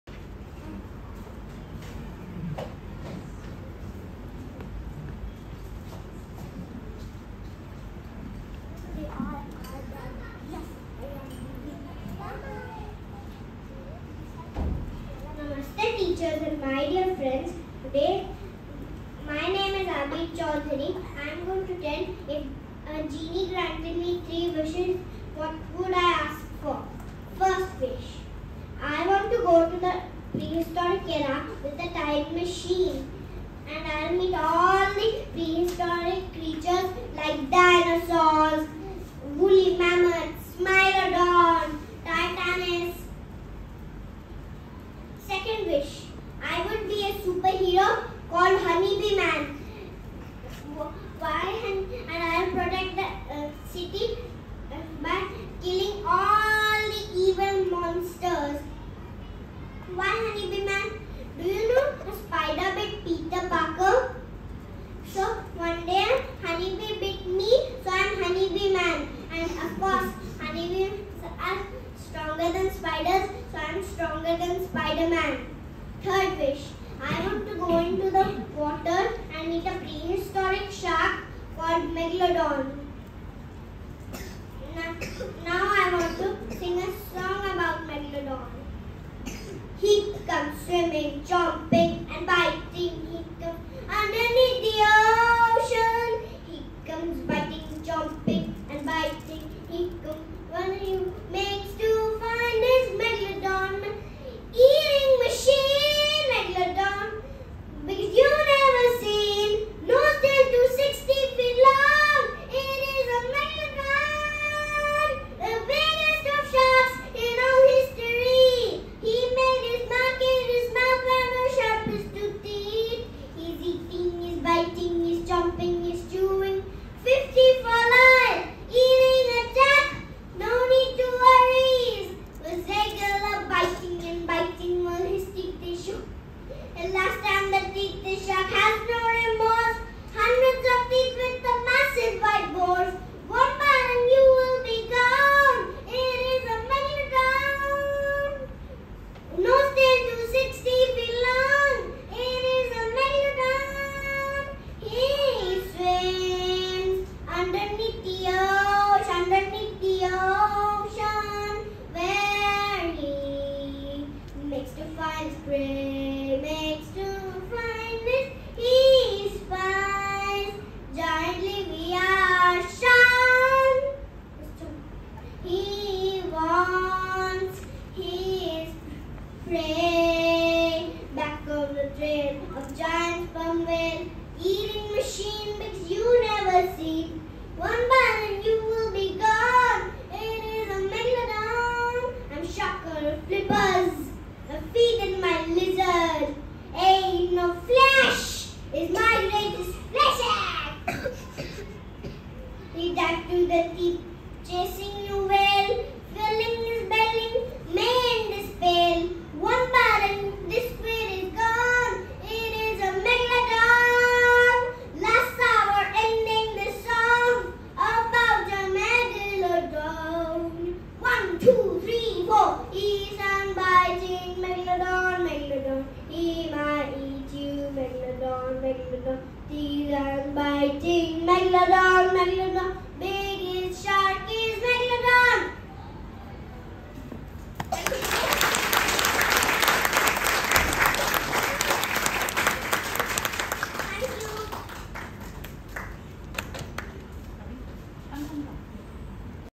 Aabeer Chowdhury Performing on Stage of Arya Vidya Mandir (Juhu)
This was Aabeer’s first stage performance in a school event called My Expressions where they had to express their feelings on a particular topic given. His topic was - if a Genie granted me 3 wishes, what would I ask for.
So his wishes were as follows:
1. Going to the prehistoric era and meeting his favourite creatures like the dinosaurs, wooly mammoth, smilodone and Titanis.
2. He would want to be a super hero called Honey Bee Man (he has mentioned his own reason in the video to call himself that) and save the world from evil monsters.
3. He would go deep under the see and find a prehistoric shark called Megalodon.
Since a lot of us might not know much about Megalodon, Aabeer sings a song to explain about that shark.
Beautiful day spent in his school where all the kids performed beautifully on stage.